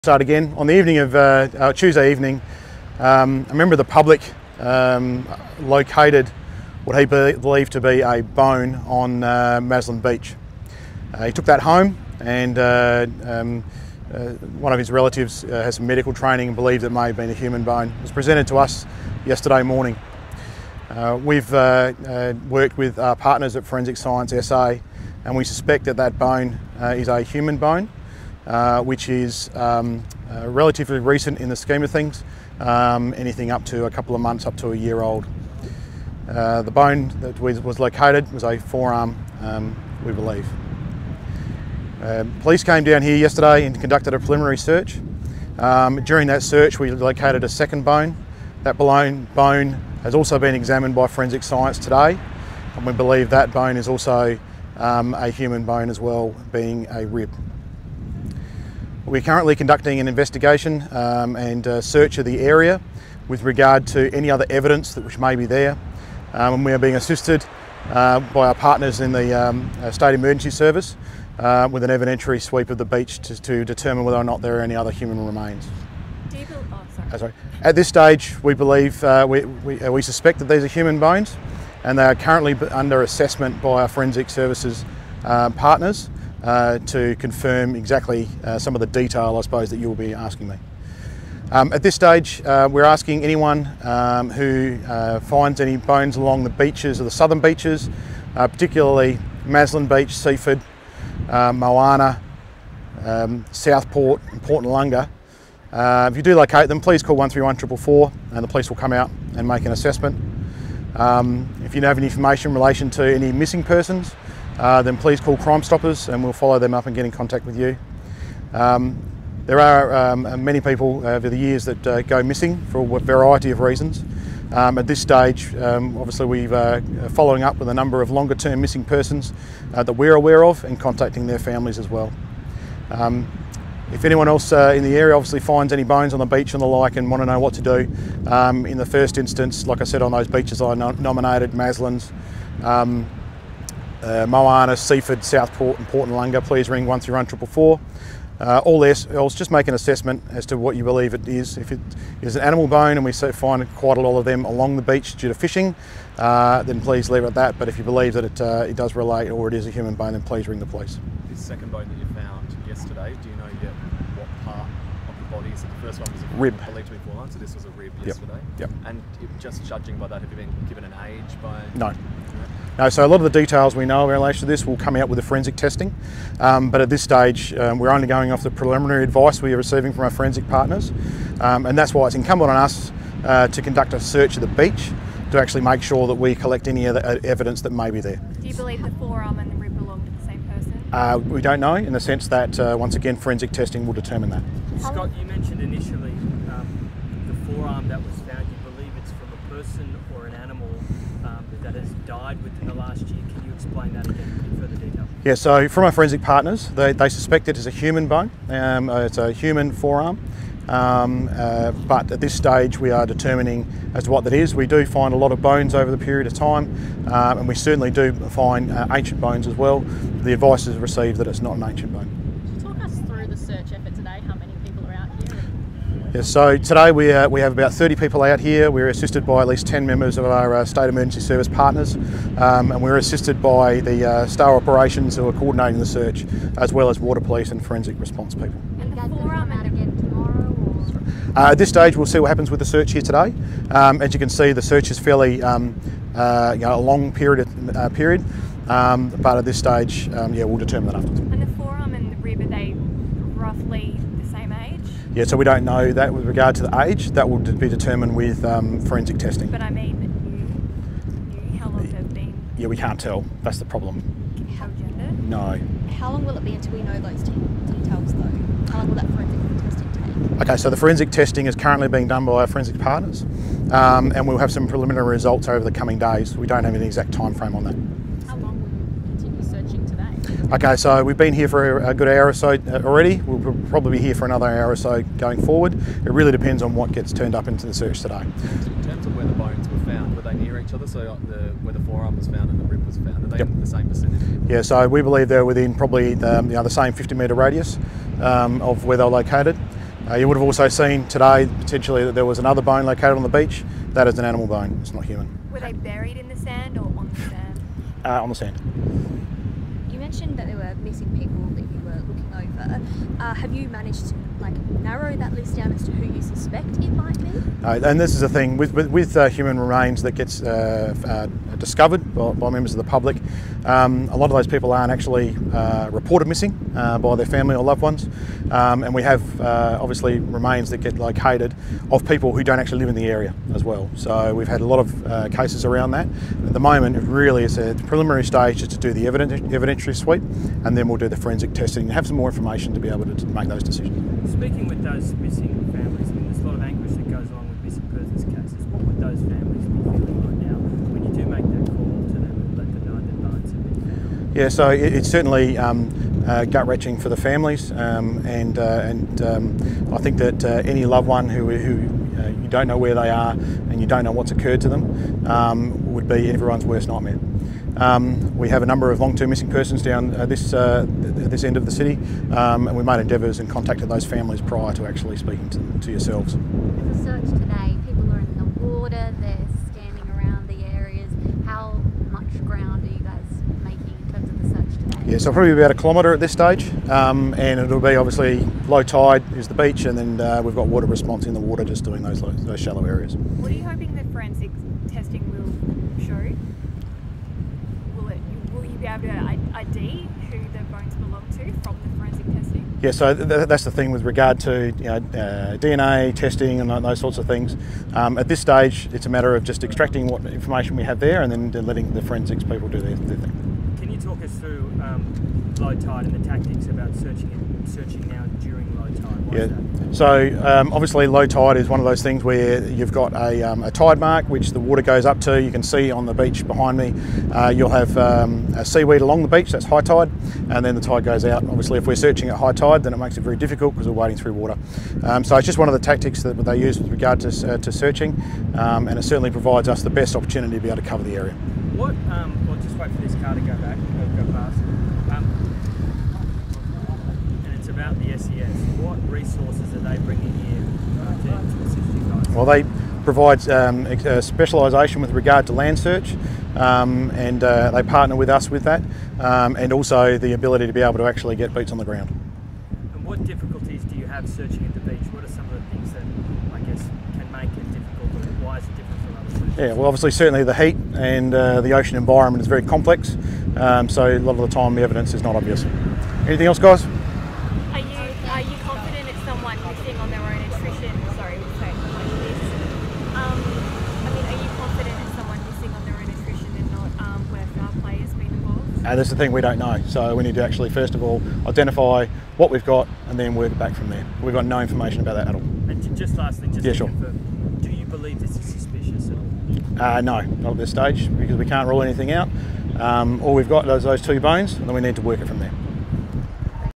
Bones found at Maslin Beach
Police and SES volunteers are conducting a search along Maslin Beach after what is suspected to be a human bone was found on Tuesday night.

A member of the public contacted police after locating the bone on Tuesday evening 16 February, while walking on the beach.

The bone has since been identified as possibly a human bone, with further forensic testing being conducted.

Southern District Police are investigating with the assistance of SES volunteers are searching the Maslin Beach area today for any other bones or signs of human remains.

Police are reviewing all existing missing persons.  Anyone with information on the incident is asked to please contact Crime Stoppers on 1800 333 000.